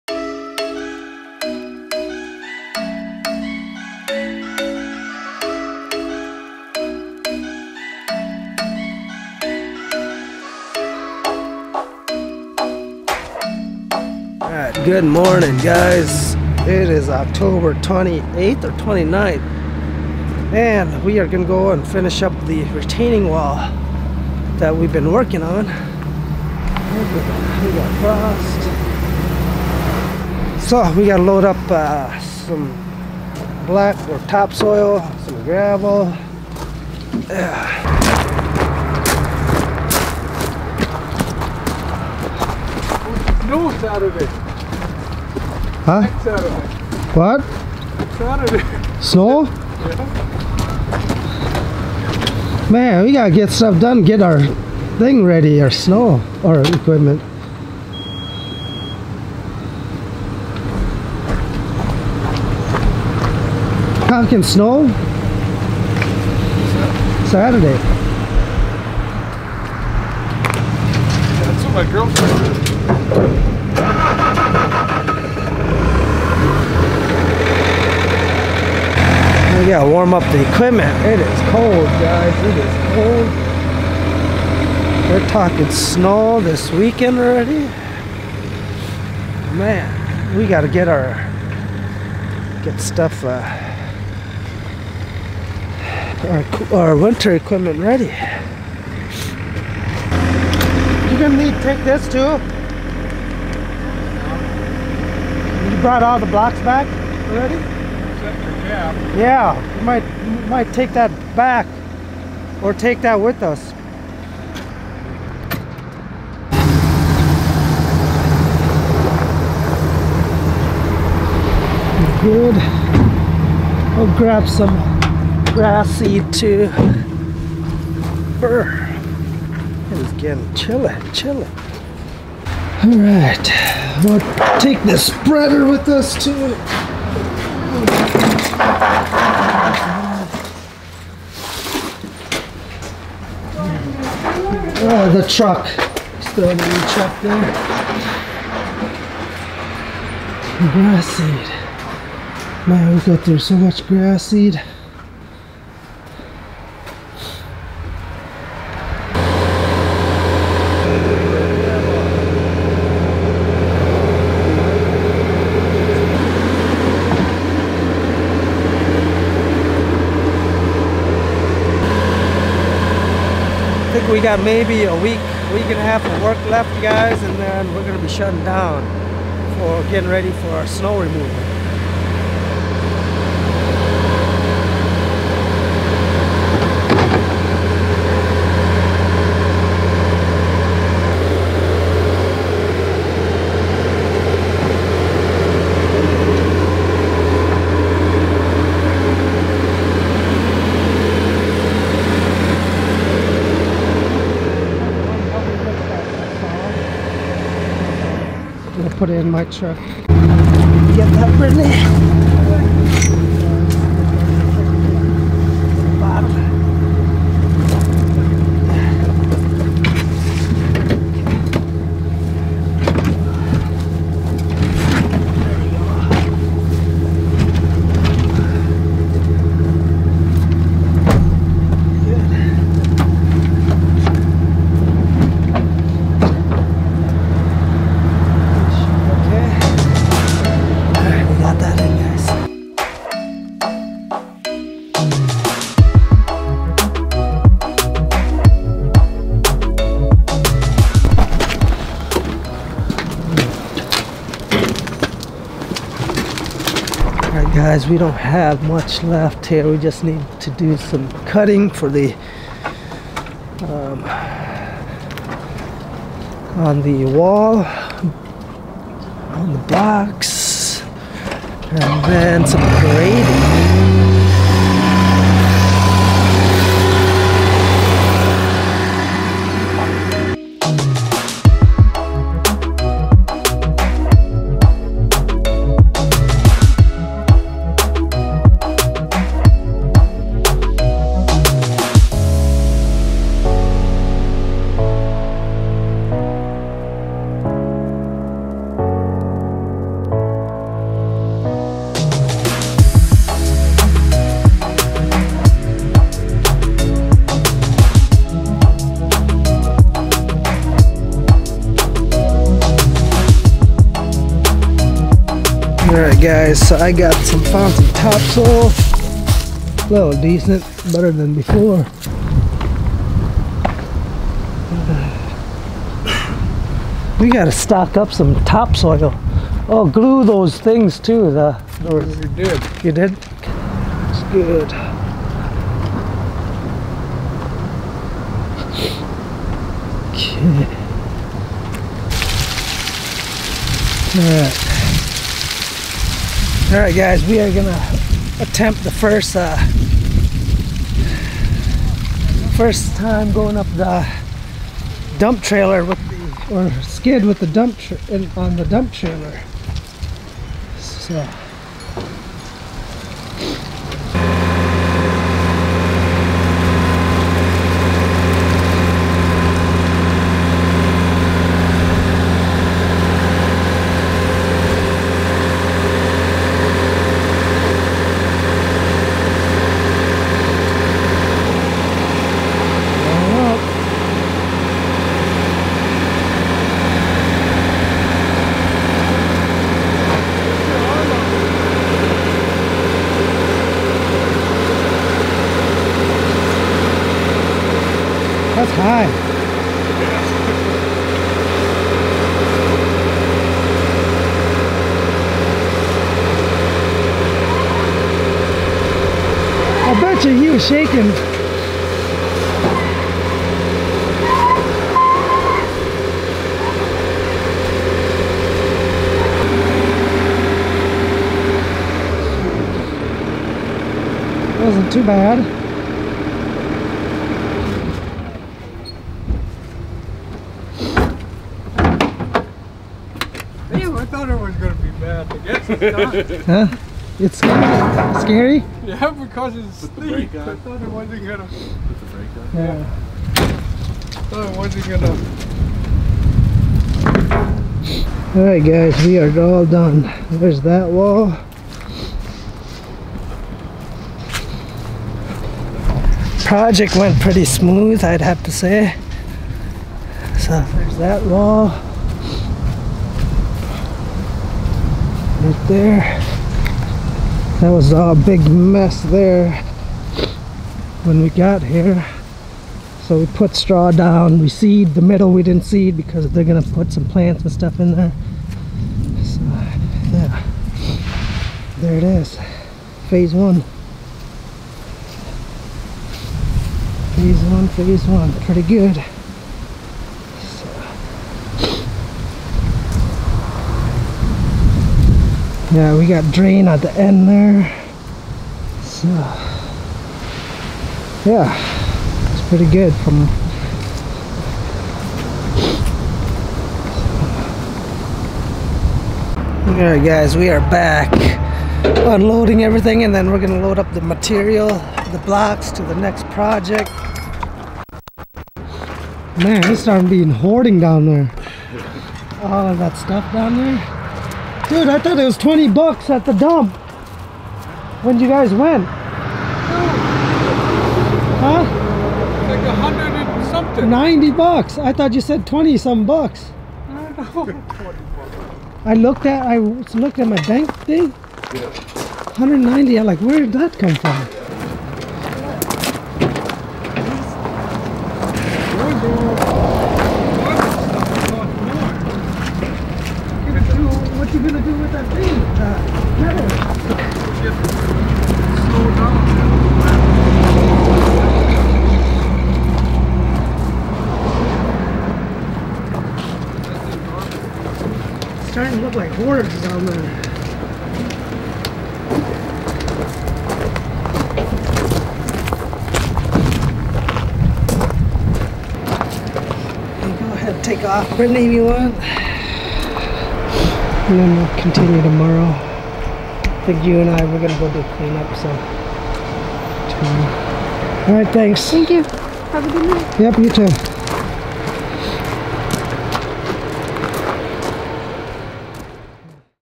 Loading a BobCat S595 on a Dump Trailer Retaining wall project complete
MailingAddress